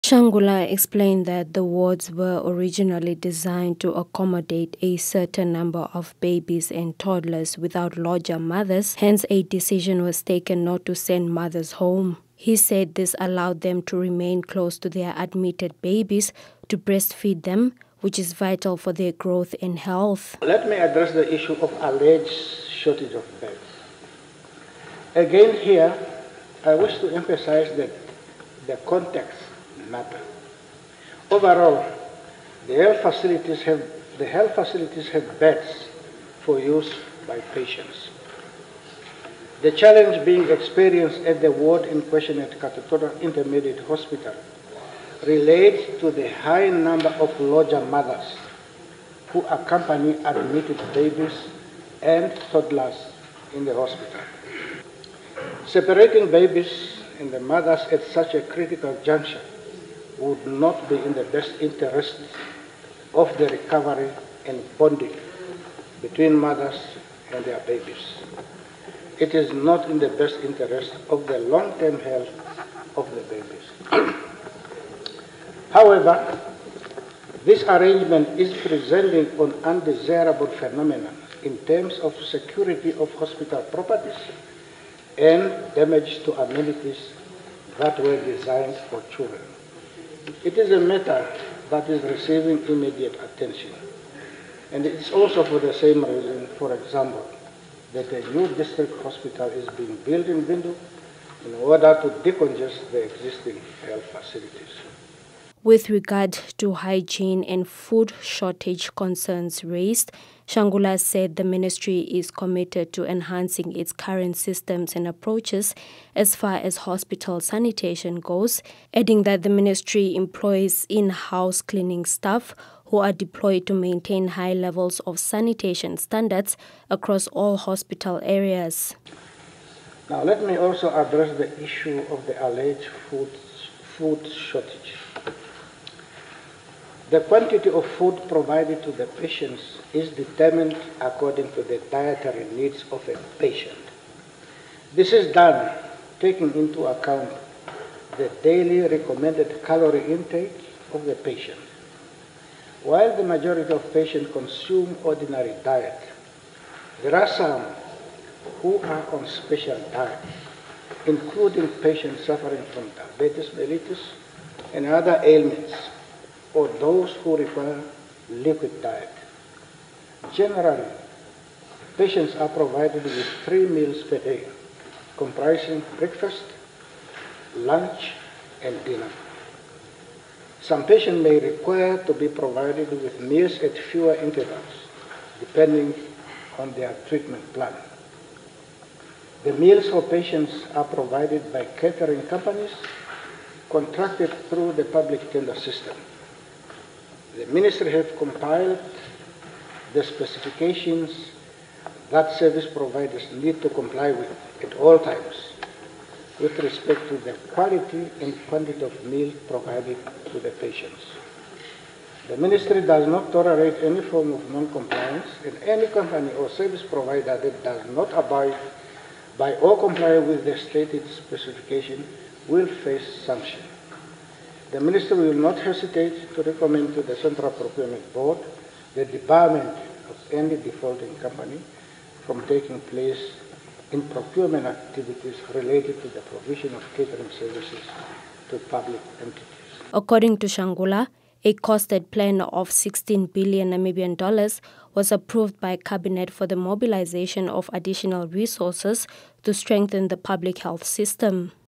Over-crowding by lodger mothers at Katutura Hospital a concern - Shangula
By: Selma Taapopi

The Minister of Health and Social Services Dr. Kalumbi Shangula says the issue of over-crowding by lodger mothers at the Katutura Intermediate Hospital is receiving immediate attention from his Ministry.

Shangula said this in response to concerns over the high number of lodger mothers who accompany their admitted babies and toddlers to the hospital.

The health minister was responding to the Landless People’s Movement (LPM) chief whip in the National Assembly, Utaara Mootu's concerns with regards the overall state of the health facility.

Shangula explained that the wards were originally designed to accommodate a certain number of babies and toddlers without lodger mothers, hence a decision was taken not to send mothers' home.

He said this allowed them to remain close to their admitted babies to breastfeed them, which is vital to their growth and health.

"Now let me address the issue of alleged shortage of beds, again here I wish to emphasize that the context matters.

Overall, the health facilities have beds for use by patients. The challenge being experienced at the ward in question at Katutura Intermediate Hospital relates to the high number of lodger mothers who accompany admitted babies and toddlers to the hospital.

Separating babies and the mothers at such a critical juncture would not be in the best interest of the recovery and bonding between mothers and their babies, it is not in the best interest of the long-term health of the babies.

However, this arrangement is presenting an undesirable phenomenon in terms of security of hospital properties and damage to amenities that were designed for children, it is a matter that is receiving immediate attention" the Minister explained.

With regards to the hospital hygiene and food shortage concerns raised, Shangula said the Ministry is committed to enhancing its current systems and approaches as far as hospital sanitation goes adding that the Ministry employs in-house cleaning staff who are deployed to maintain high levels of sanitation standards across all hospital areas.

According to Shangula, a costed plan of N$16 billion was approved by the cabinet for the mobilization of additional resources to strengthen the public health system.

The funds will be allocated towards the renovation, expansion, and modernization of physical infrastructure, recruitment of health professionals, and procurement of medical equipment and ambulances among other interventions.

Watch on oneafrica.tv DStv or GOtv at 19h00 on Mondays to Fridays.